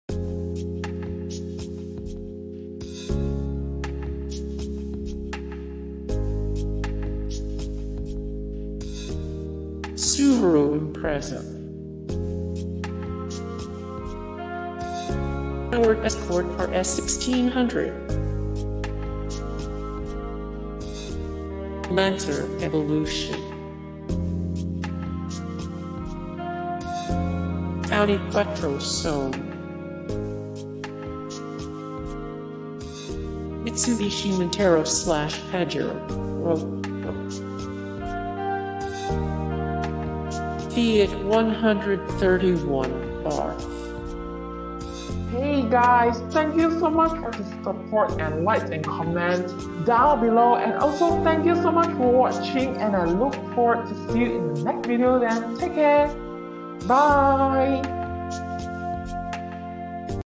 The Best Rally Cars Ever Put Together ✅
Purists are always going on about "real" motorsport, but can a motorsport be any more "pure" than one that predates paved roads? Rally racing is probably the oldest form of motorsports on Earth, and it's seen some incredible machines in its time. Without a doubt, most of them came from the early 1980s - a time of turbocharged Group B monsters and their truly suicidal drivers. While people who care about little things like death ultimately outlawed the most insane of all racing classes, we've seen quite a few greats since then.Some of best rally cars have come from the WRC, but let's be real: since Group B went away, it's been impossible to shake the fact that WRC cars are intentionally much slower than they could be. The same isn't true in the Paris-Dakar rally, or on Pikes Peak. In the desert wilds and mountain heights, insanity still rules and speed still means going as fast as possible. For this list, we're going to look over some of the driving greats from all three series. For the record, we're excluding Pikes Peak vehicles from 2011 and later, since a bunch of San Francisco hippies forced Colorado to pave Pikes all the way to the top. Loss of a national treasure, that was. But we've certainly seen our share of rally treasures sliding sideways through the years. Time to kick some gravel. Vote up the good rally cars you wish you could see race.Photo: flickr/CC0...more
0:00 - Intro
0:00:08 - Subaru Impreza
0:00:15 - Ford Escort RS1600
0:00:22 - Lancer Evolution
0:00:29 - Audi Quattro S1
0:00:36 - Mitsubishi Montero/Pajero
0:00:44 - Fiat 131 Abarth